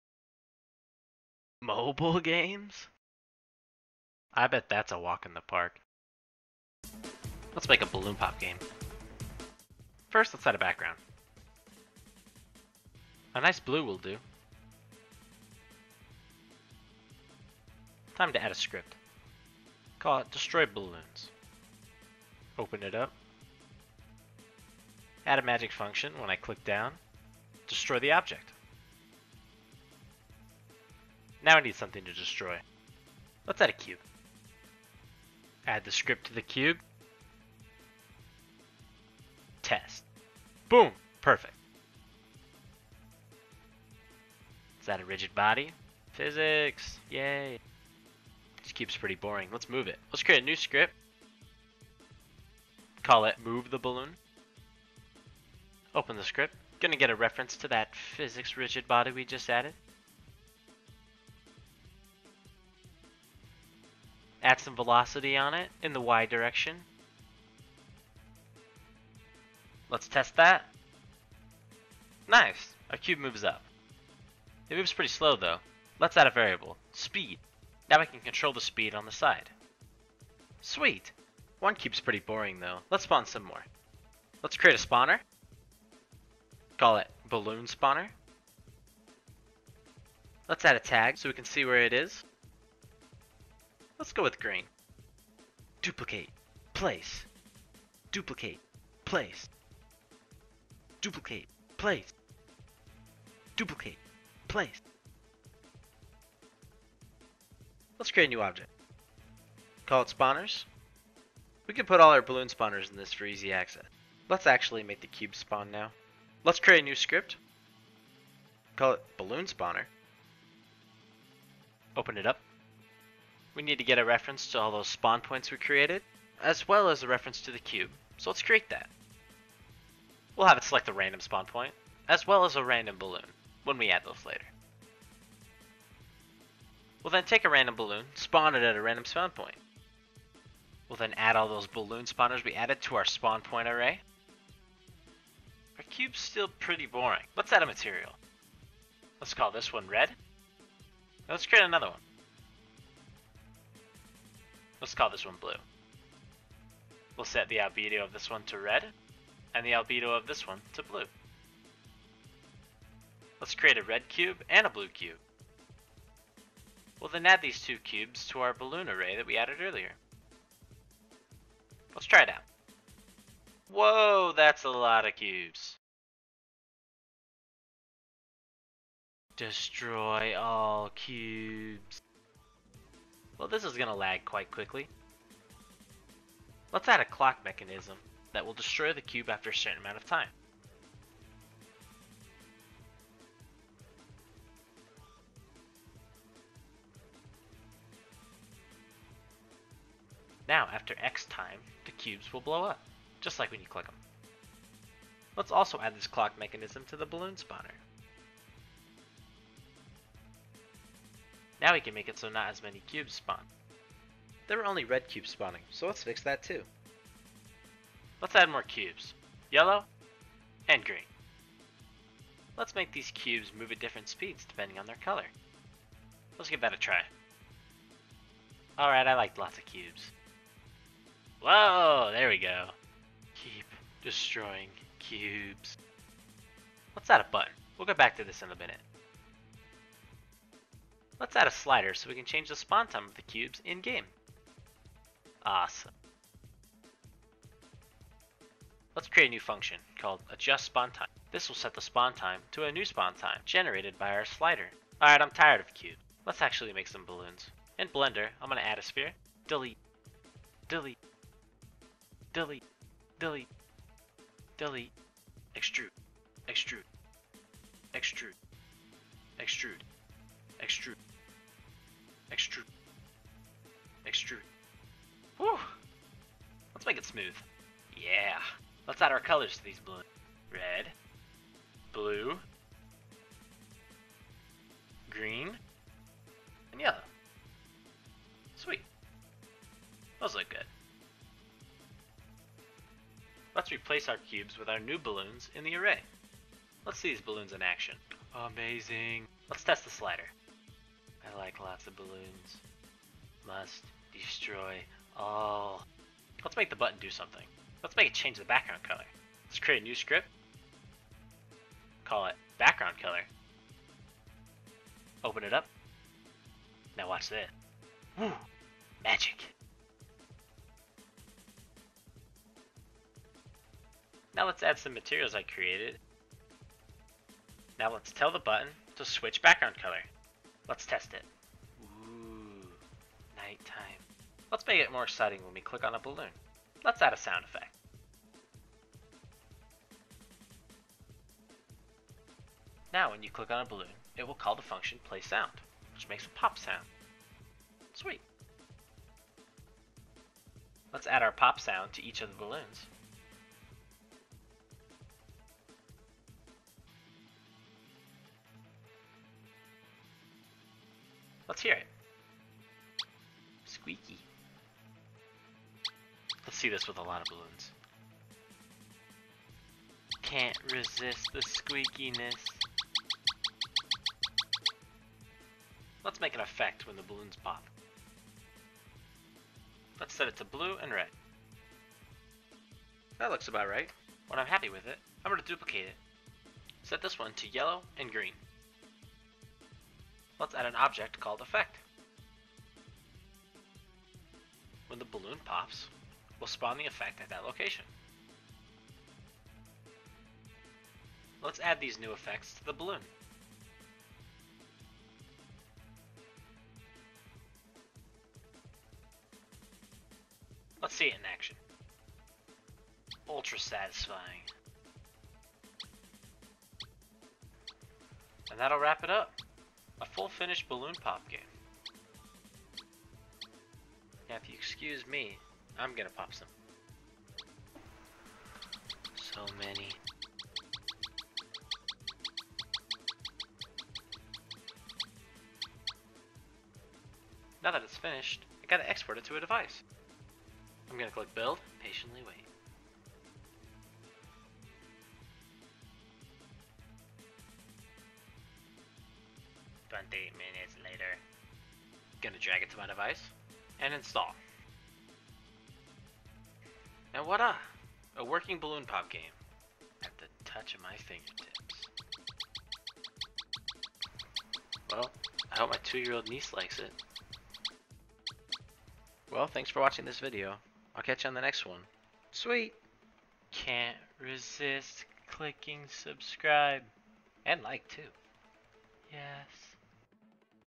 How to make a Mobile Game! - Balloon POP!
Follow me along as I make a children's game in Unity, designed for Mobile devices.

Check me out on Twitter!